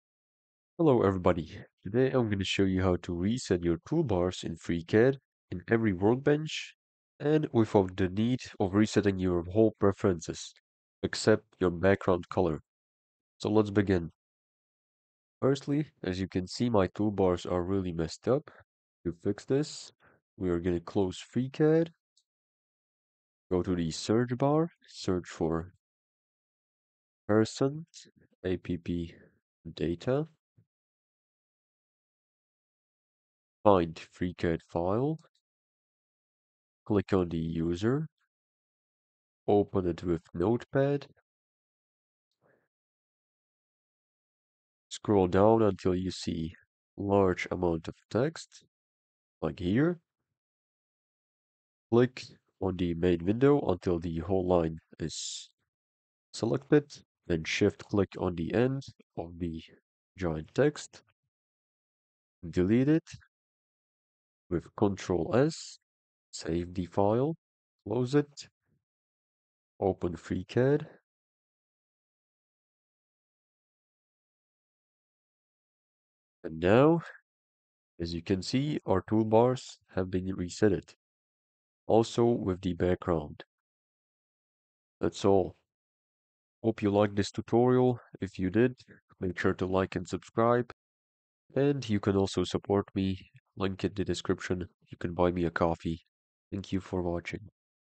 How to Reset Toolbars In FreeCad
📌 How to Reset Toolbars in FreeCAD (1.0+) | Quick Fix Tutorial
Having trouble with missing or misplaced toolbars in FreeCAD? In this quick tutorial, I’ll show you how to reset your toolbars to their default layout.

✅ Works with FreeCAD 1.0 and newer
💡 Simple, safe, and effective